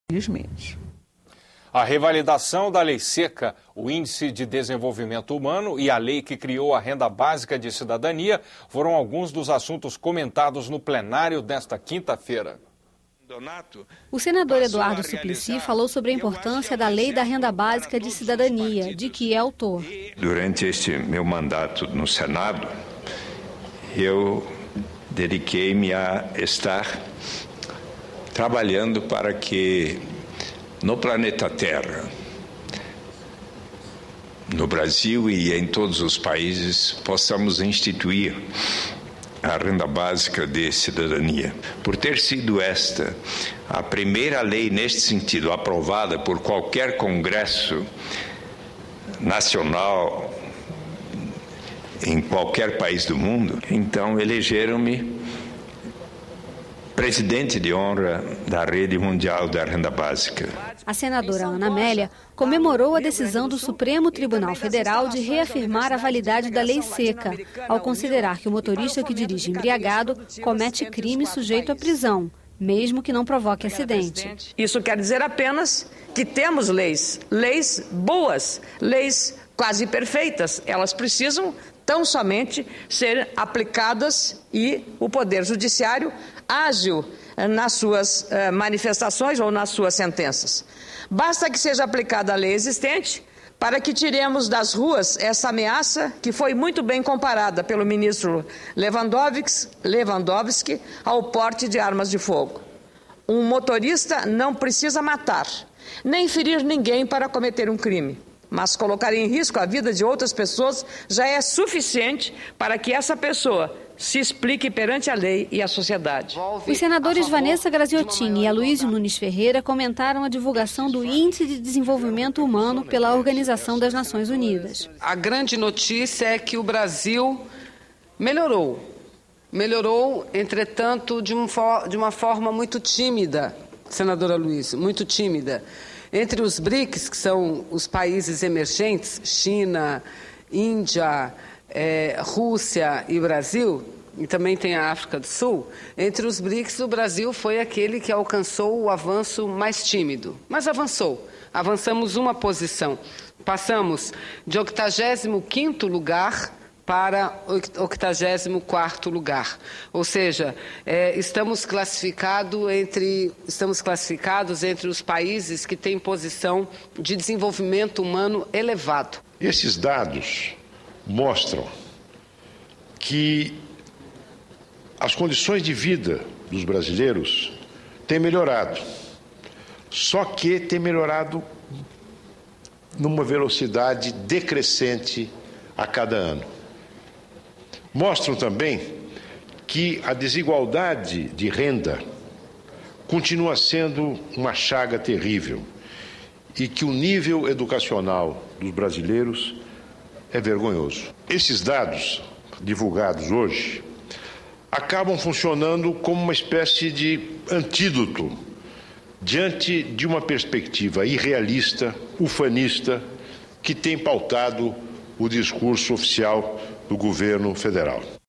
Jornalismo - Plenário desta quinta-feira - Bloco 1
A revalidação da Lei Seca, o Índice de Desenvolvimento Humano  e a lei que criou a renda básica de cidadania são alguns dos assuntos comentados no plenário nesta quinta-feira.